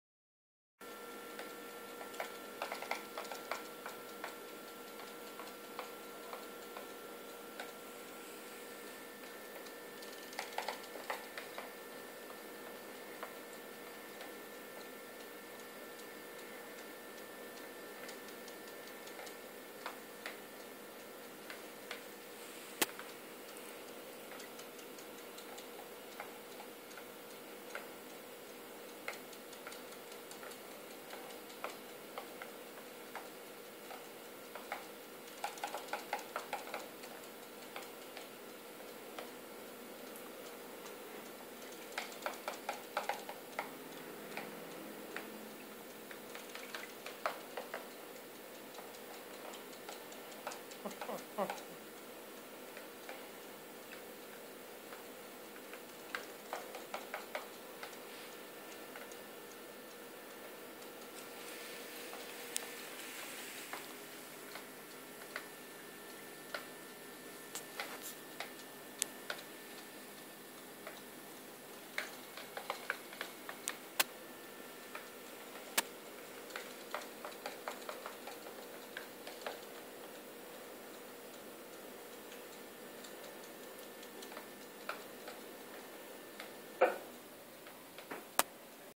Electronic Fish Play Toy For Ragdoll Cats (Canadian Link Included)
Tasa and Toby Ragdoll Cats play with their robot Fish for the first time. My two are not crazy about getting their paws wet, but they love to watch the fish.